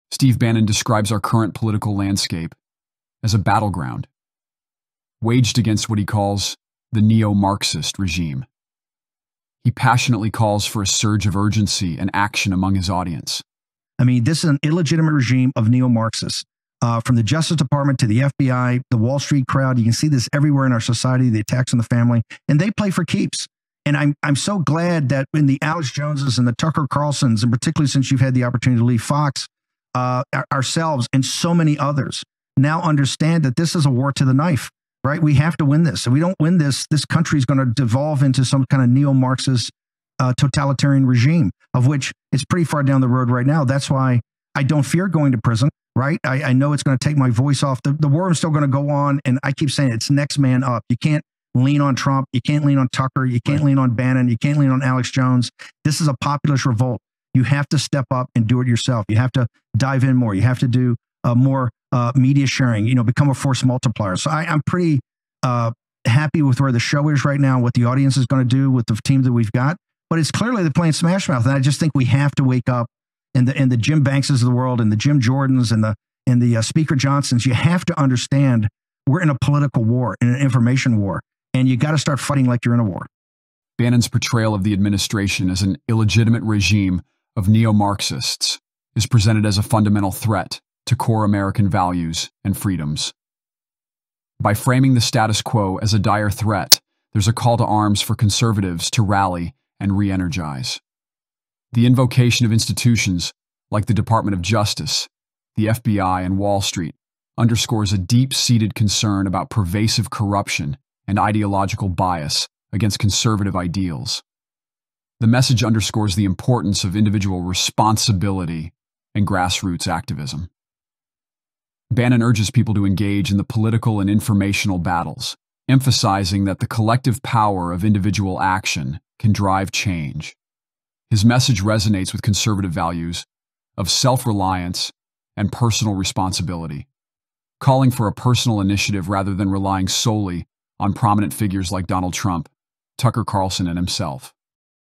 Steve Bannon, We Have to Wake Up... We're in a Political War
Steve Bannon warns Tucker Carlson: "Wake up, we're in a political war." In this explosive interview, Bannon outlines the threats facing America and the urgent need for action. He describes the current regime as a Neo-Marxist entity aiming to dismantle traditional values and freedoms. Bannon emphasizes the importance of understanding this as a battle for the nation's future, urging individuals to step up, share media, and become force multipliers in this struggle. Join us as we delve into Bannon's powerful insights and the imperative to fight like we're in a war to protect our liberties and way of life.